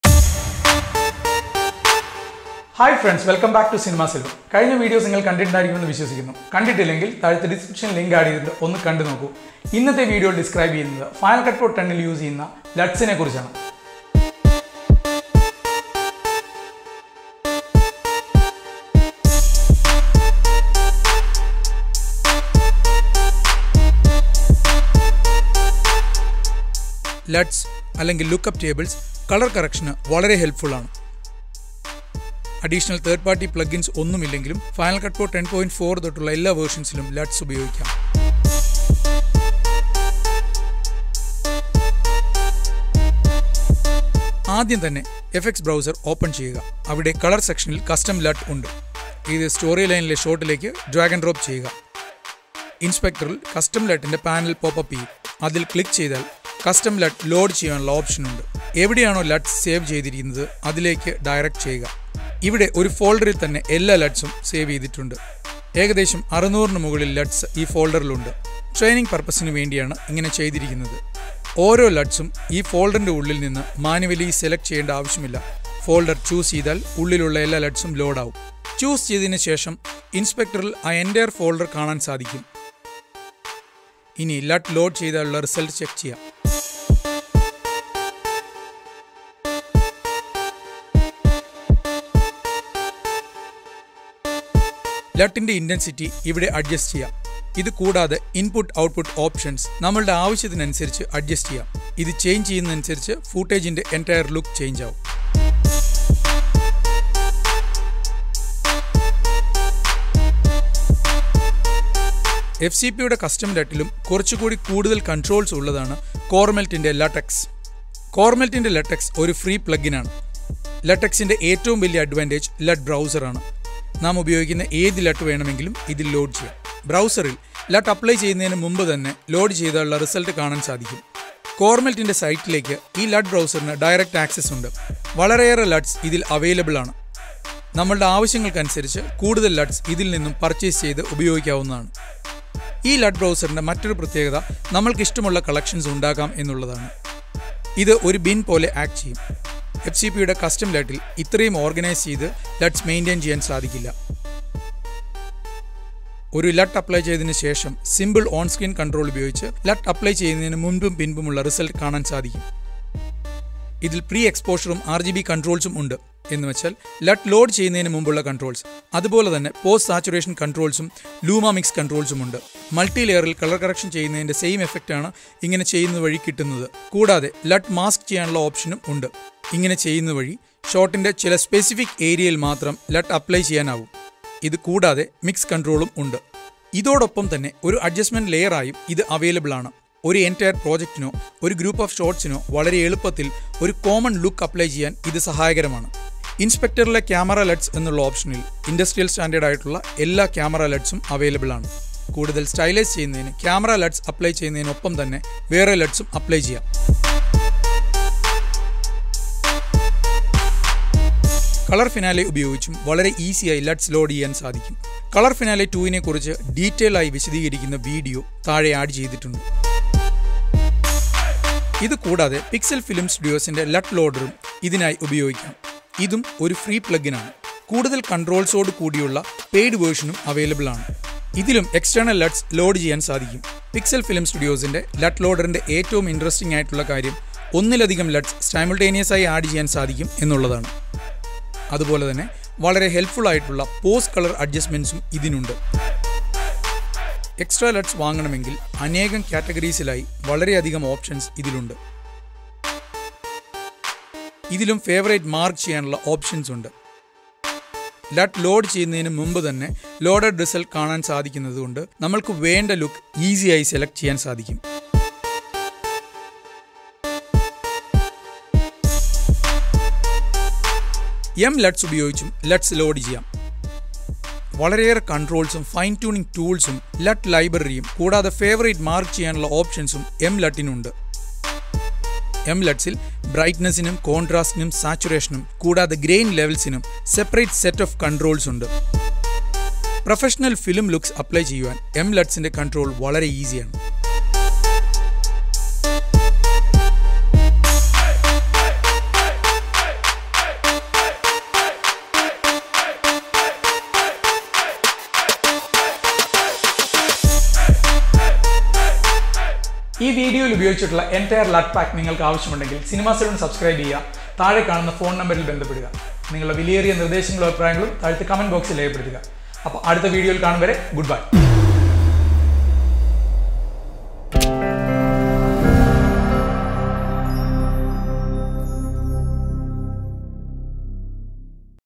LUTS in FCPX(മലയാളം) finalcutprox malayalam tutorial
Sreekumar Nair, Colourist & Apple Certified Pro, Cinema Salone, explains about using LUTs (Look Up Table) in FCPX.